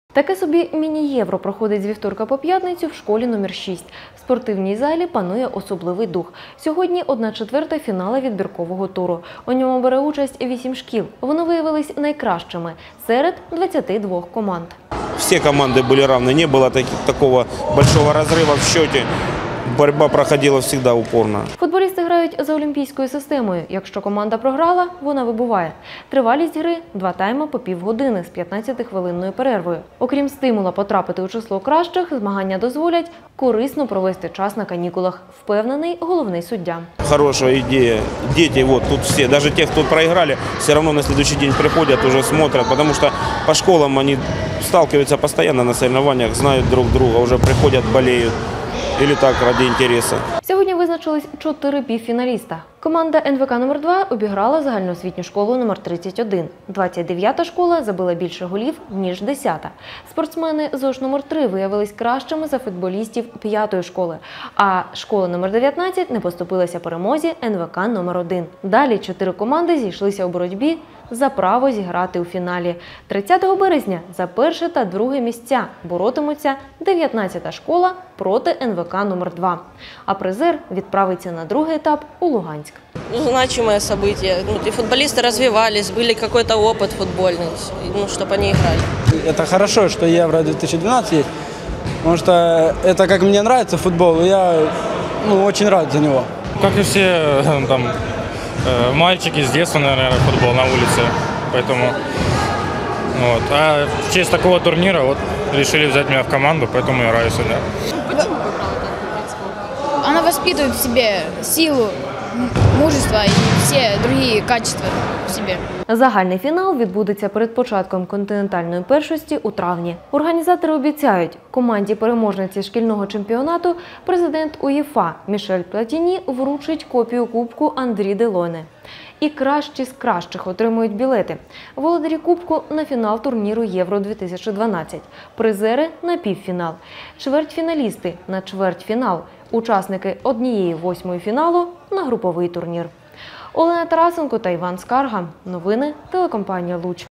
Шаг в большой спорт. В борьбе за поездку на "Евро-2012"
Обычные ворота, мяч и форма. Необычные стимул и идея. Школьники всей страны в последние дни марта и первые дни апреля соревнуются в Школьном чемпионате по футболу "Евро-2012". Кроме громкого названия, он имеет специальные условия игры и особенное вознаграждение. Решающий матч турнира пройдёт на национальном спортивном комплексе "Олимпийский". За победу будут соревноваться больше 12 тысяч учебных заведений. На полях посёлков, городов, районов, столицы, будут играть свыше 300 тысяч школьников. За играми краснолучских команд наблюдала наша съемочная группа.